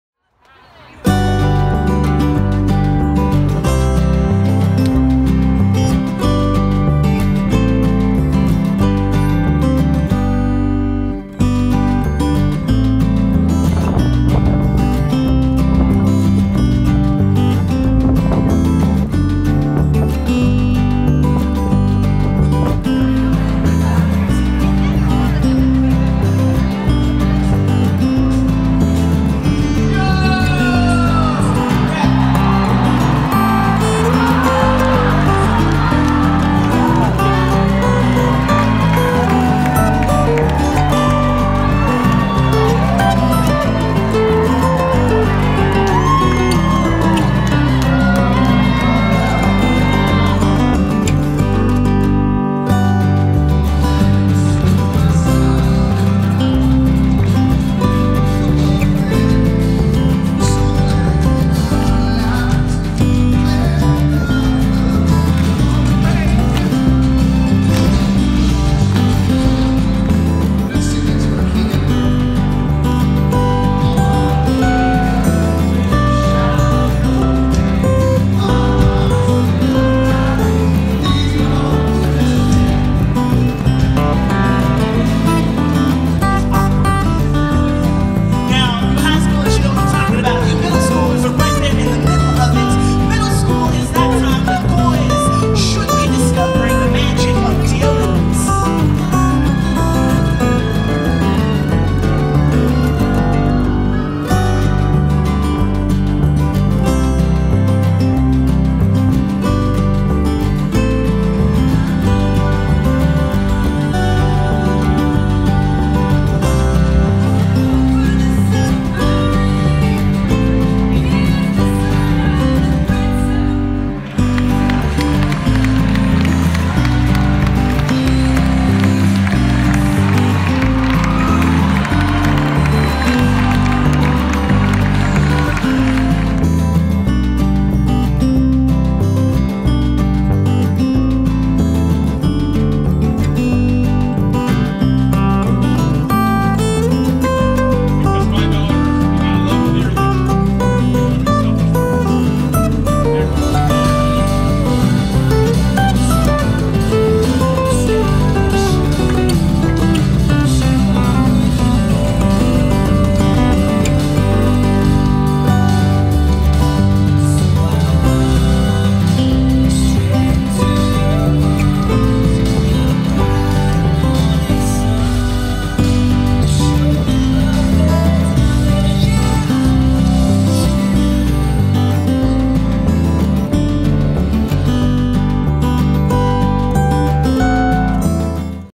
ROCK Retreat 2019
HYPE Youth Ministry's trip to ROCK Retreat 2019, an event for the youth of the Baltimore-Washington Conference of the United Methodist Church.